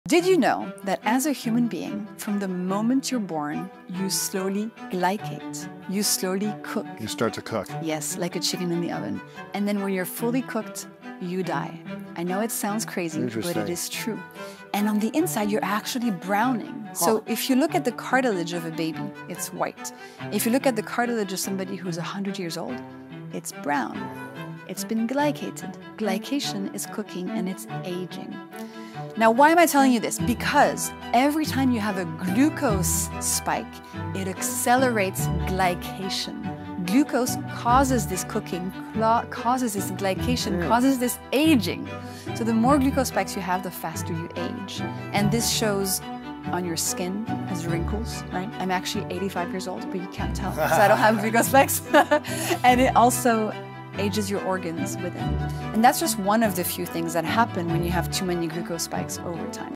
Glucose Spikes Are Giving You Wrinkles | Jessie Inchauspé
Original Video:

In this fascinating video, Jessie Inchauspé shares a groundbreaking perspective on aging and health that might change the way you see your diet and lifestyle. Jessie explains the concept of glycation, a process where our bodies "cook" from the inside as we age, likening it to a chicken roasting in an oven. This process, Jessie reveals, is significantly accelerated by glucose spikes in our body, which can lead to visible signs of aging like wrinkles and also affect the health of our internal organs. If you're intrigued by the science of aging and want practical tips on how to slow down this "cooking" process, this video is packed with insights.

Jessie dives into the importance of managing glucose spikes to combat the aging process. By understanding how our dietary choices impact our body's aging, viewers can learn how to make healthier decisions that may not only improve their skin's appearance but also enhance their overall wellbeing. Jessie's approach to aging and health is both innovative and enlightening, offering a new angle on why maintaining stable glucose levels is crucial for longevity and vitality.

By the end of this video, you'll be equipped with the knowledge to make more informed choices about your diet and its impact on aging. Jessie Inchauspé provides a compelling argument for paying attention to how our bodies react to food, especially in terms of glucose spikes. Whether you're looking to improve your skin health, extend your lifespan, or just feel better in your day-to-day life, Jessie's insights on glycation and aging are an invaluable resource. This video is a must-watch for anyone interested in the intersection of nutrition, health, and the science of aging.